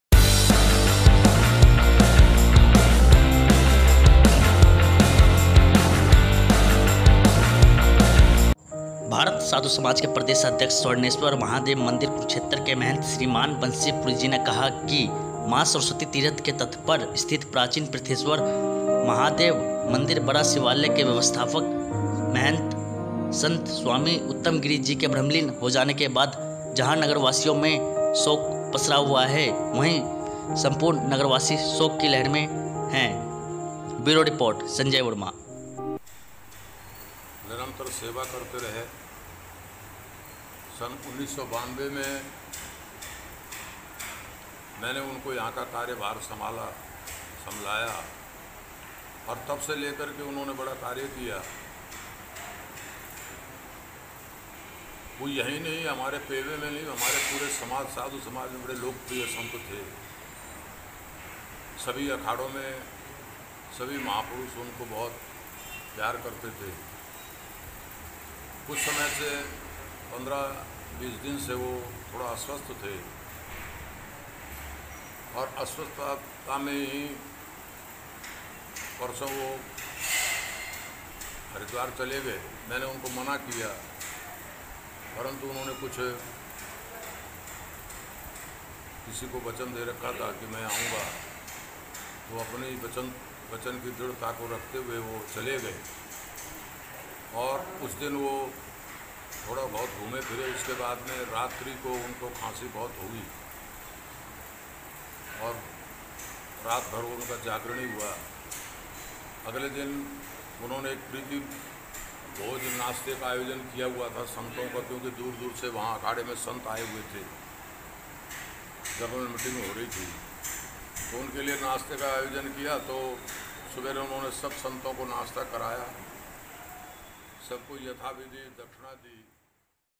प्राचीन पृथ्वेश्वर महादेव मंदिर बड़ा शिवालय के व्यवस्थापक संत स्वामी उत्तम गिरी जी हुए‌ ब्रह्मलीन
स्टोरी :- प्राचीन पृथ्वेश्वर महादेव मंदिर बड़ा शिवालय के व्यवस्थापक संत स्वामी उत्तम गिरी जी के ब्रह्मलीन होने पसरा शोक
एंकर:- भारत साधु समाज के प्रदेशाध्यक्ष, स्थाणेश्वर महादेव मंदिर कुरूक्षेत्र के महंत श्री मान महंत बंसी पुरी जी ने कहा कि मां सरस्वती तीर्थ के तट पर स्थित प्राचीन पृथ्वेश्वर महादेव मंदिर बड़ा शिवालय के व्यवस्थापक संत स्वामी उत्तम गिरी जी के ब्रह्मलीन हो जाने से जहां नगरवासियों में शोक पसरा हुआ हैं वहीं साधु समाज को भी बहुत बड़ा अघात पहुंचा है। ऐसे संत की भरपाई करना बहुत ही कठिन है। महंत बंसी पुरी ने कहा कि स्वामी उत्तम गिरी जी ने सन 1988 से प्राचीन पृथ्वेश्वर महादेव मंदिर बड़ा शिवालय के बतौर व्यवस्थापक के रूप में कार्य करते रहे। गत दिवस हरिद्वार की नीलधारा गंगा में उन्हें जलसमाधि दी गई। बंसी पुरी ने बताया कि स्वामी उत्तम गिरी जी कि नागा सन्यासी साधु में विशेष पहचान थी, जहां कहीं भी कुंभ मेला हो या अन्य कोई धार्मिक कार्यक्रम, स्वामी उत्तम गिरी जी सभी में अपनी विशेषताएं कायम किए हुए थे। अपना  आज नगरवासियों व दूर दाराज से आए उनके भक्तों ने पृथ्वेश्वर महादेव मंदिर बड़ा शिवालय पहुंचकर स्वामी उत्तम गिरी जी की प्रतिमा पर पुष्प अर्पित कर श्रद्धांजलि दी व उनके साथ जुड़ी अपनी यादें सांझा की। इस मौके पर महंत तरणदास, बाबा लाल गिरी, पं .बब्लू चौबे, राधे, सुरेश मौदगिल, ऋषि राम शर्मा, देव पूणर््िामा, सतीश छाबड़ा, बुद्धिराजा, विवेक पौलस्त्य, सहित भारी संख्या में श्रद्धालुओं ने उनकी प्रतिमा पर पुष्प अर्पित कर कहा कि भगवान भोलेनाथ उन्हें अपने चरणों में स्थान दें।
1.    बाईट:- महंत बंसी पुरी जी
2.  बाईट:- पं. बब्लू चौबे, सेवक